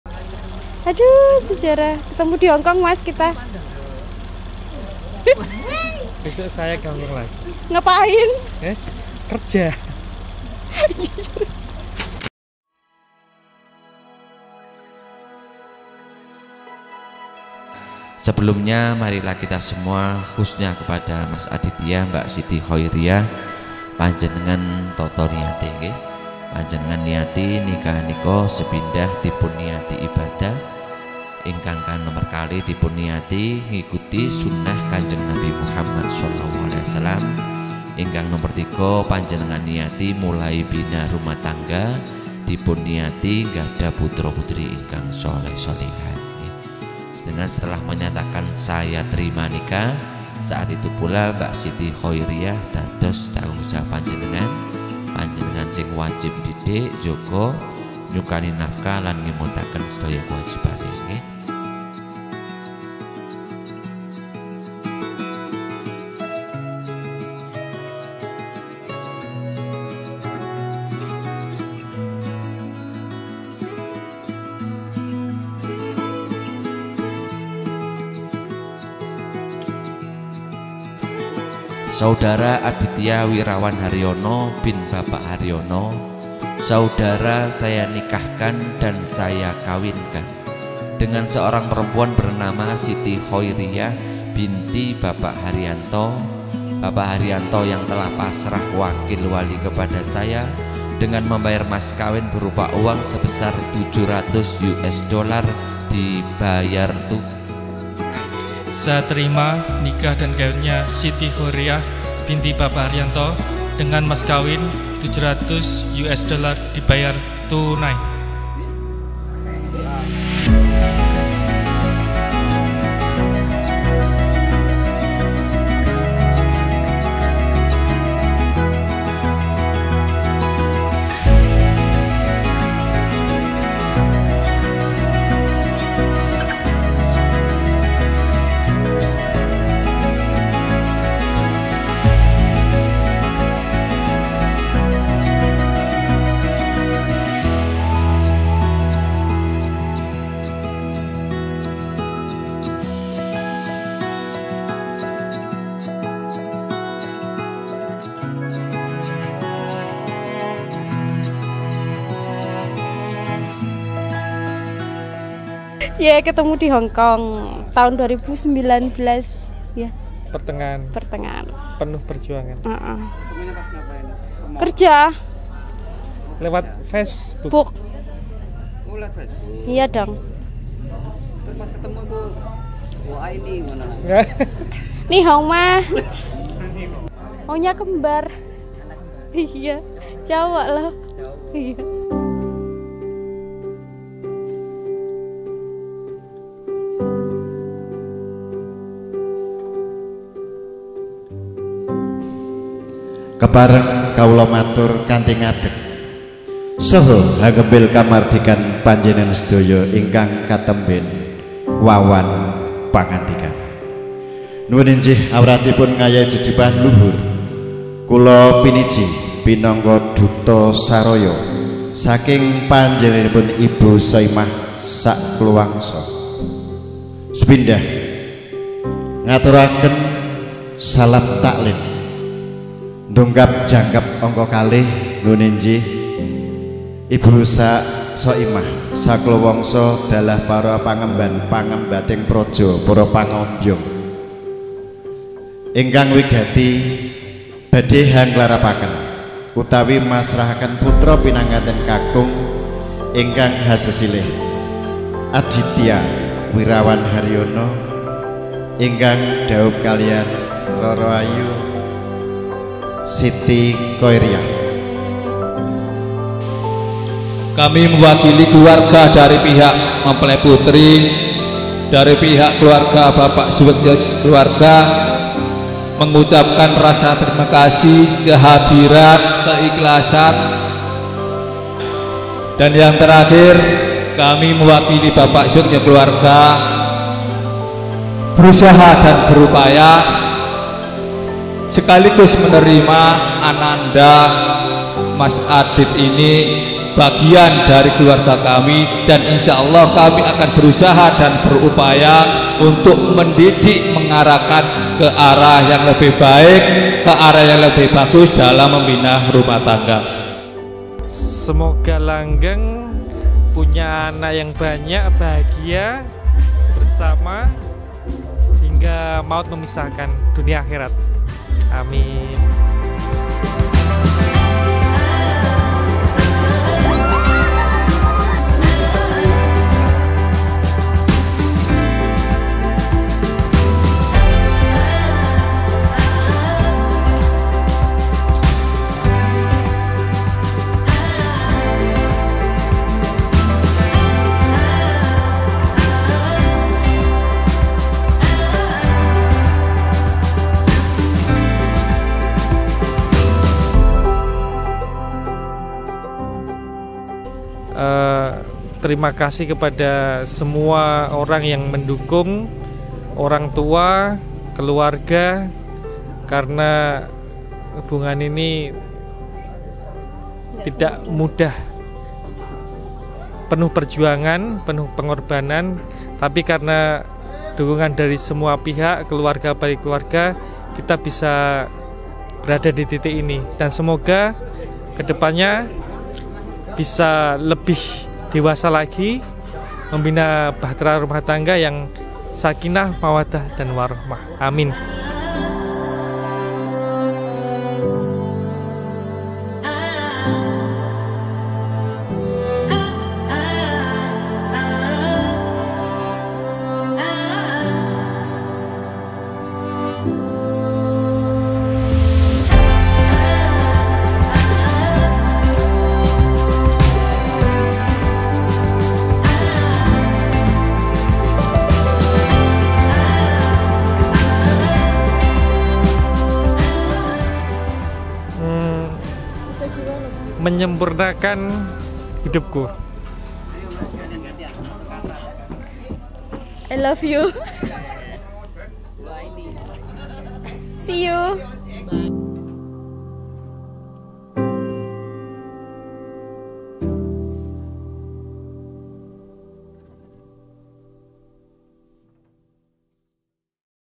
Wedding Aditya Ria (Cinematic) - Wagir, Malang (Video 2/3)
The Wedding Of Aditya & Ria
Documentation Video
Wagir - Kab. Malang
Wedding by Elita Pro

- Taken By LumixGH5 and LumixG85

Visit:
Instagram.com/almirzastudio
Instagram.com/massuv